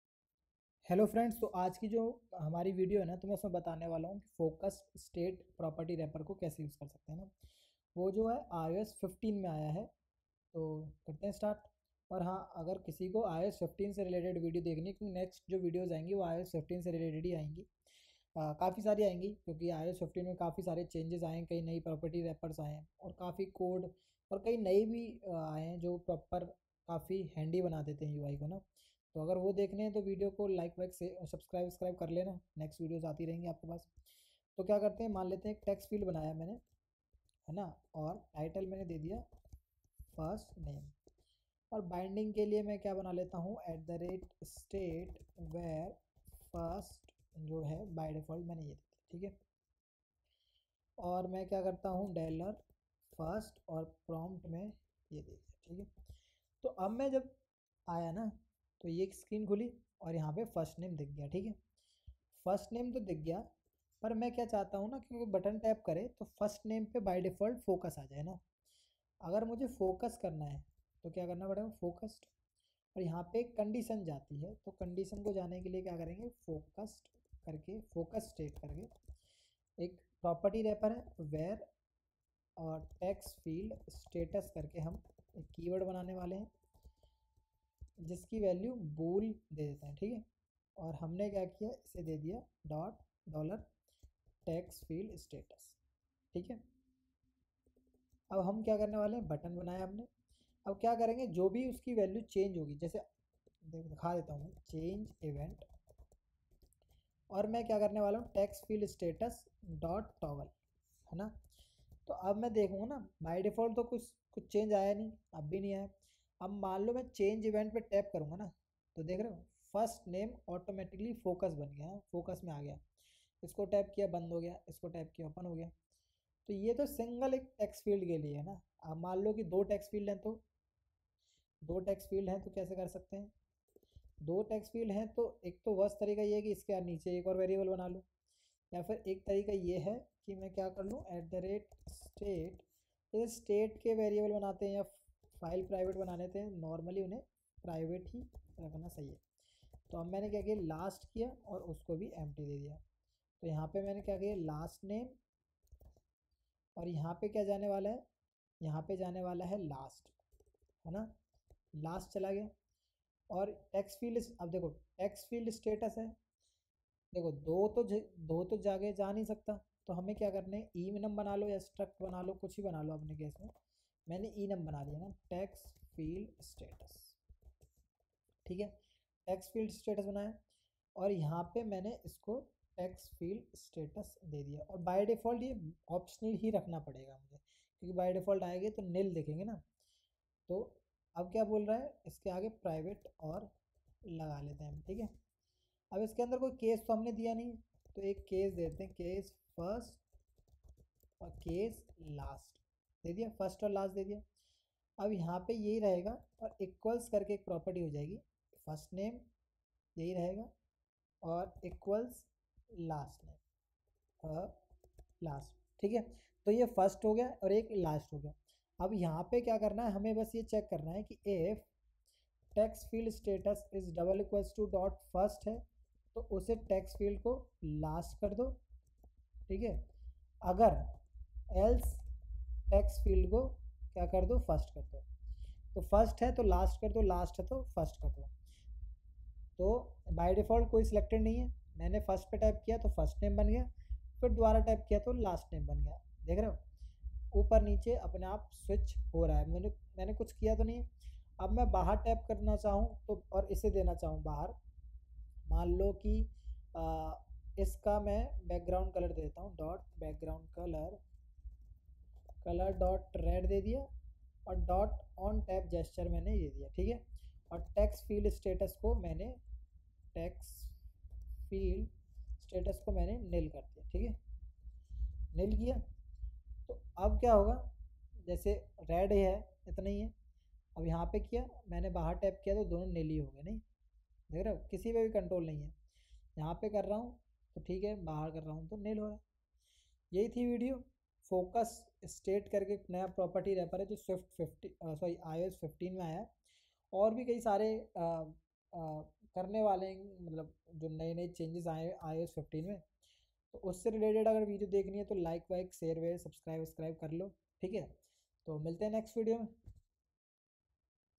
SwiftUI - @FocusState | How to use @FocusState in SwiftUI | iOS 15
Hello friends, In this video I tried to explain @FocusState in SwiftUI. Please watch till the end.